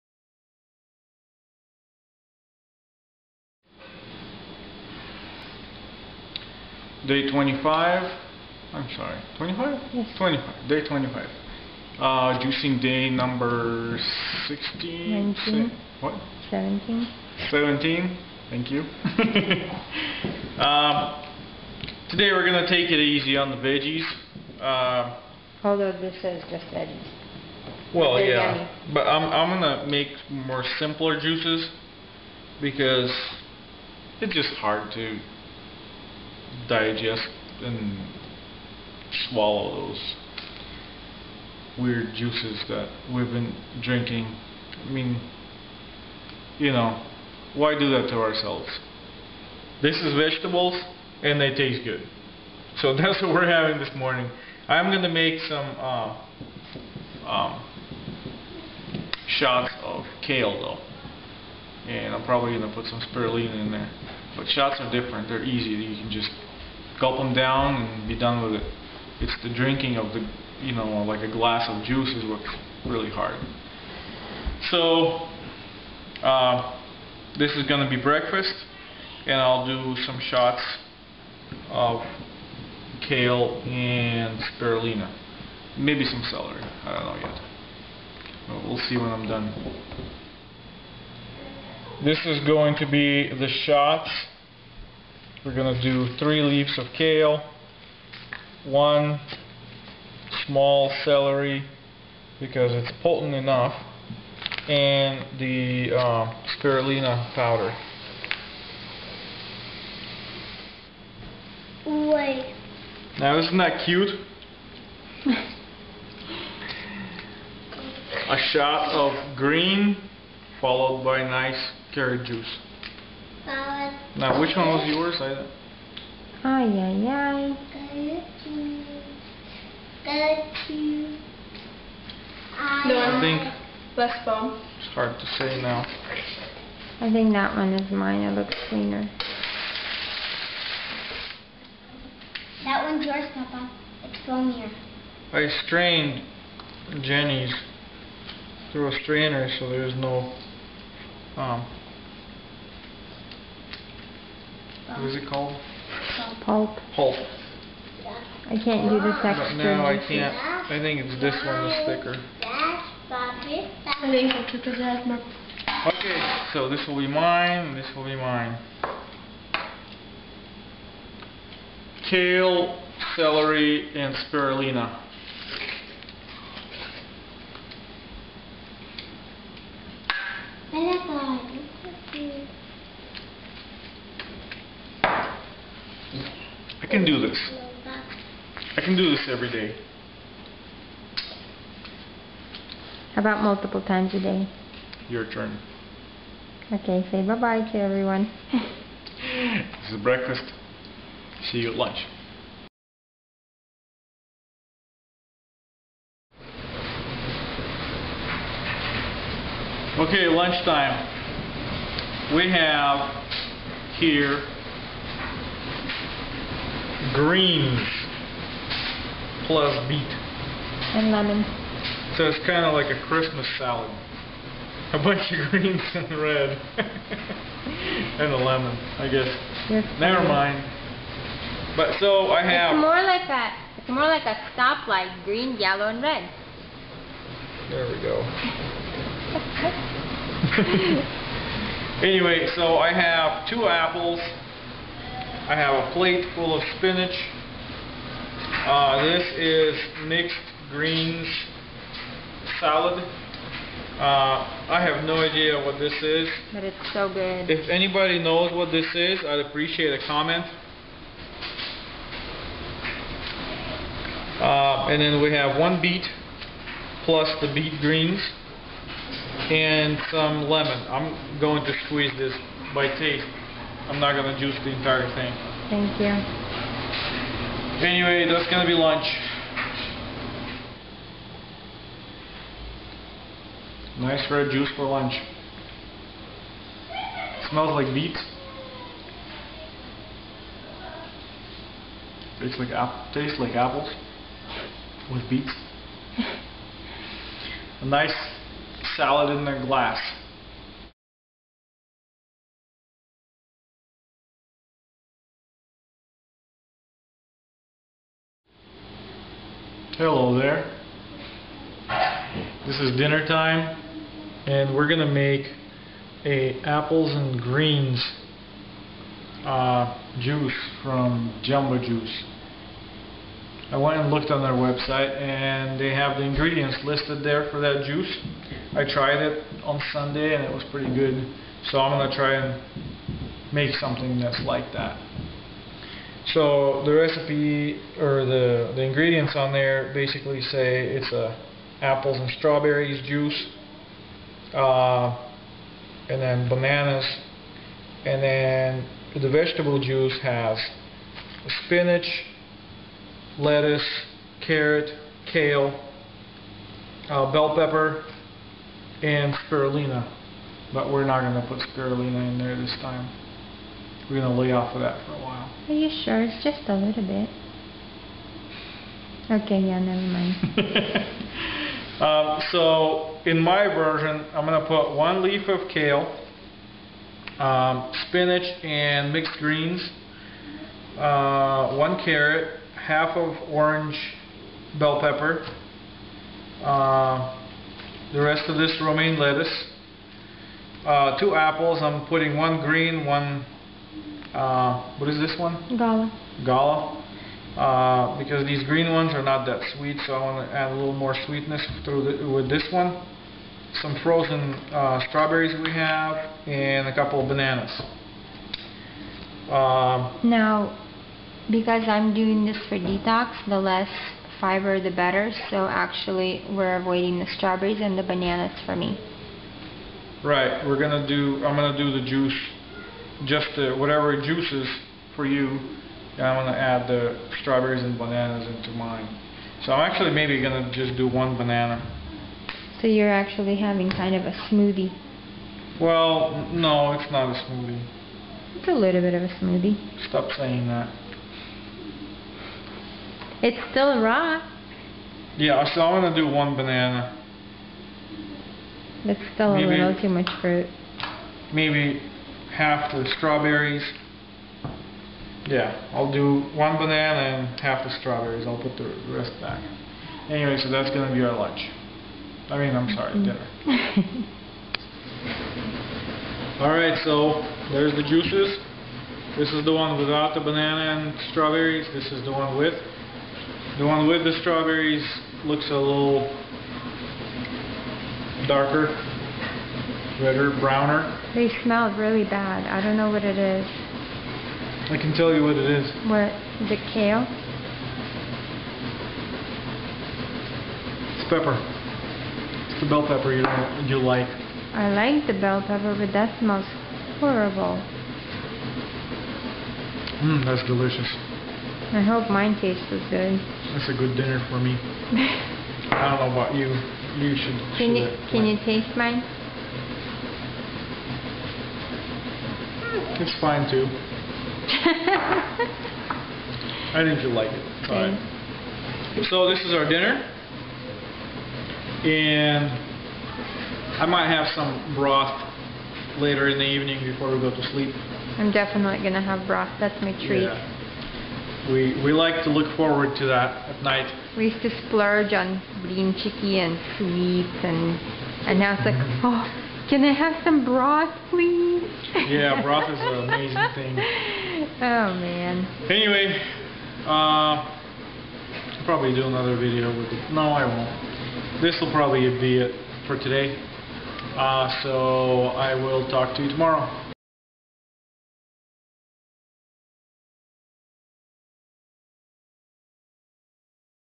Day 25: Juice Fasting & Kale Juice Shots
- Another round of kale juice shots this morning, plus we added celery and spirulina in there to give it a little kick. I also made a Apples & Greens juice from Jamba Juice.

for more juice fasting videos.